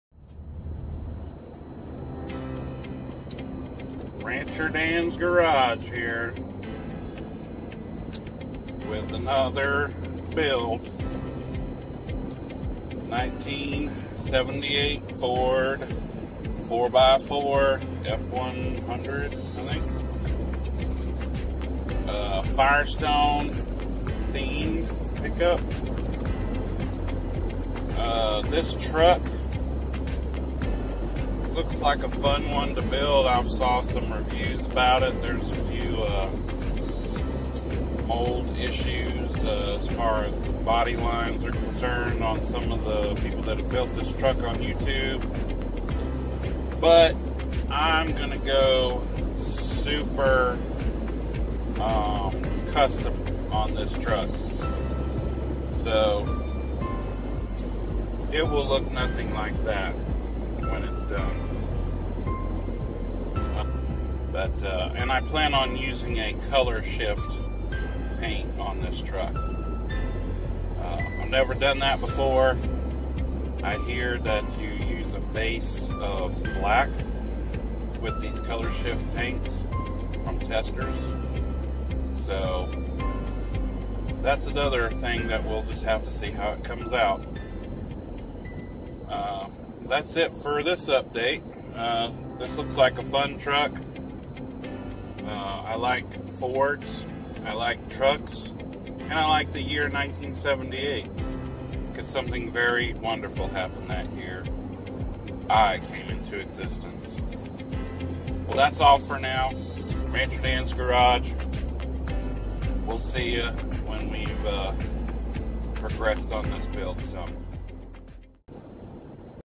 Introduction to 1978 Ford 4X4 pickup
Introduction to the next build. A 1/25th scale Ford 4x4 pickup from AMT.